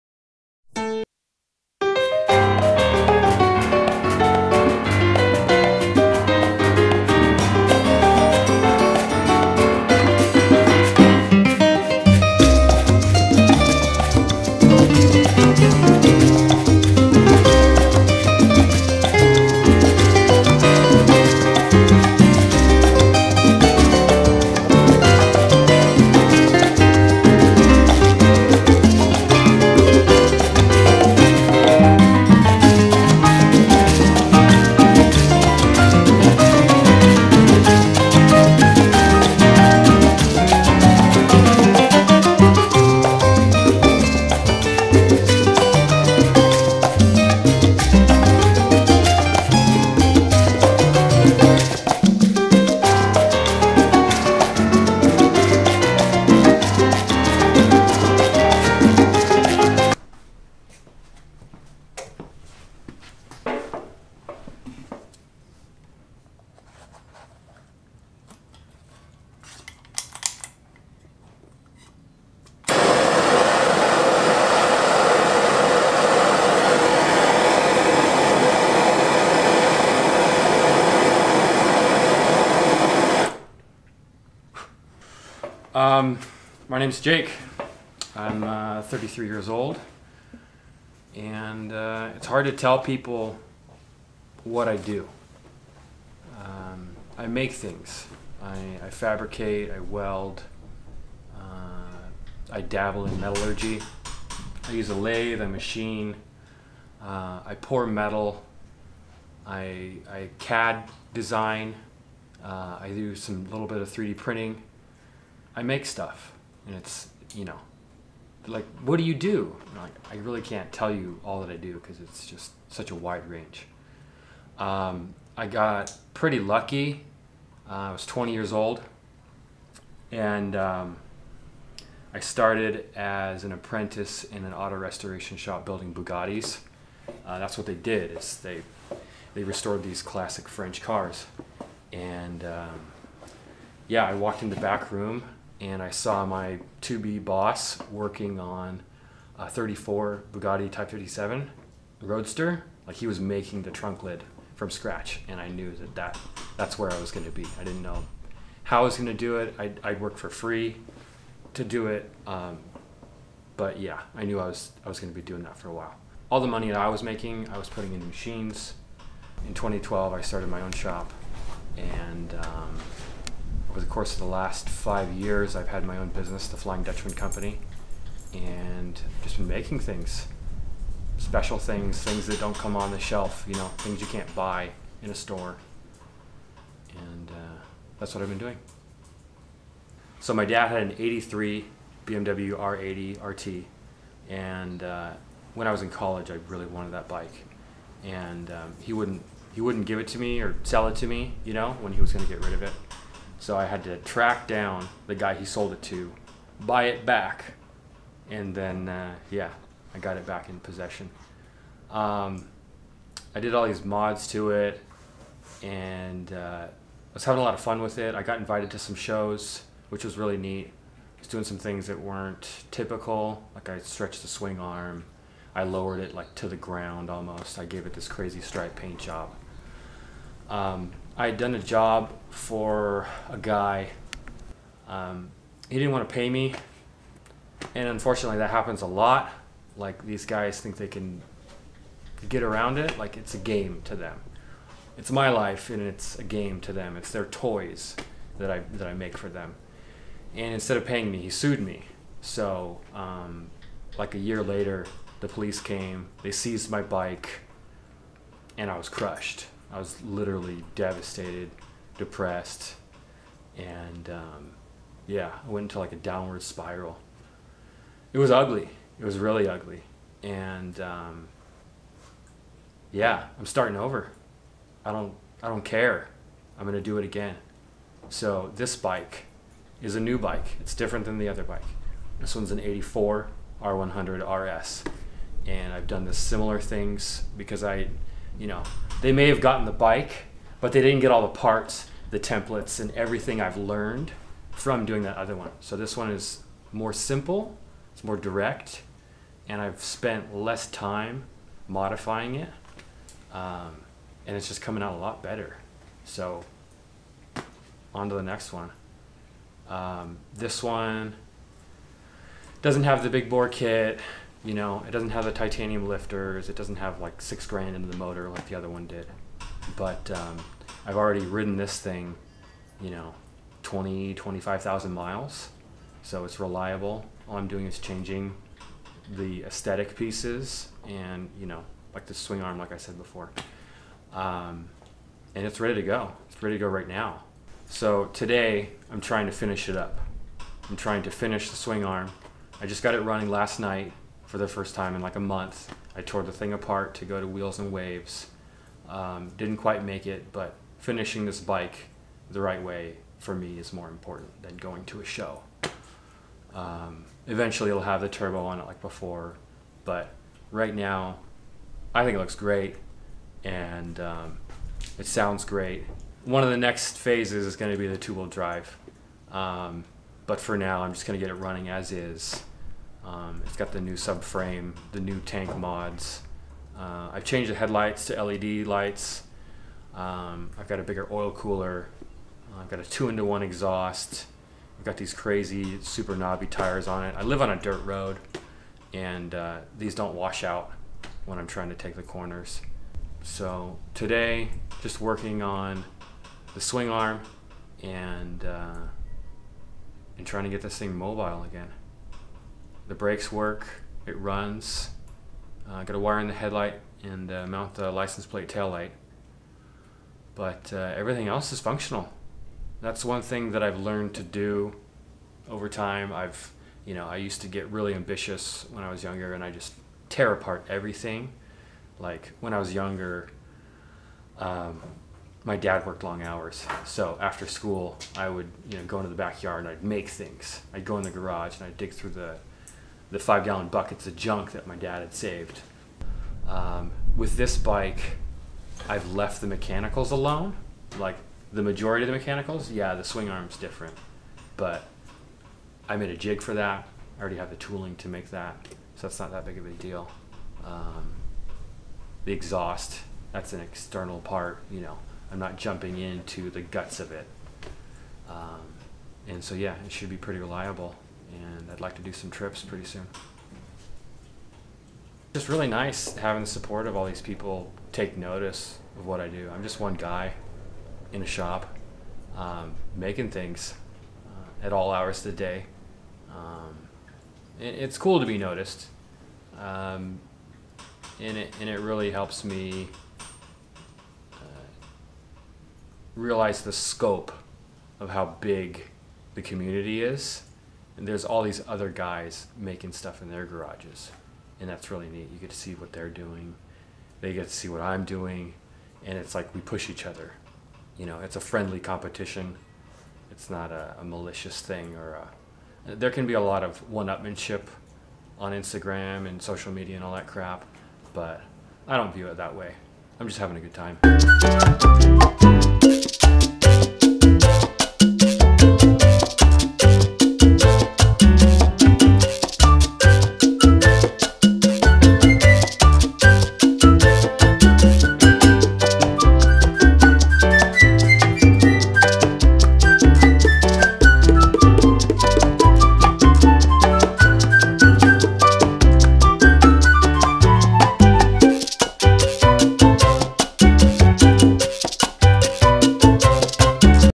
The Flying Dutchman
We spent the day in San Marcos CA with Jake Krotje of The Flying Dutchman Company while he worked on his BMW R100RS project.

Filmed and edited by Eric Konetzny